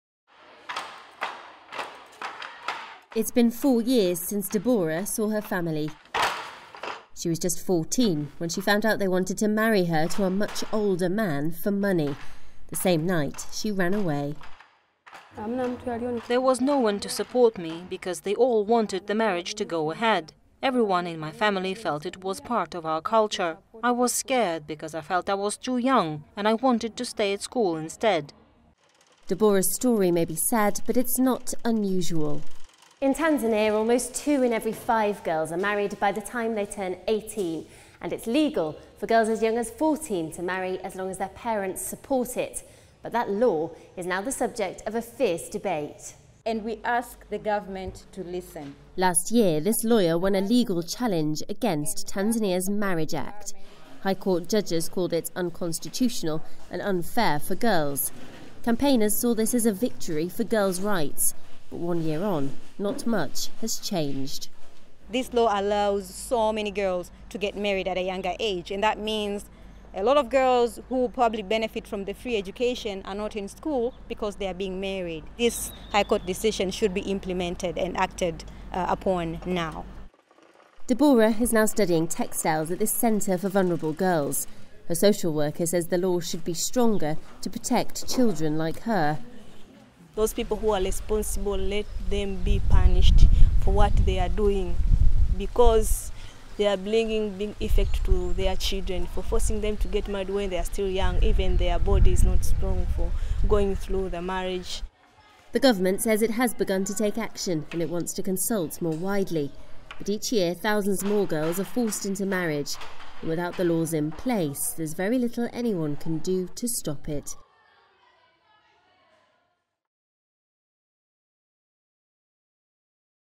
Government defies child marriage ban (2017, Tanzania)
One year on, Tanzania has yet to outlaw child marriage, despite an order by the Supreme Court.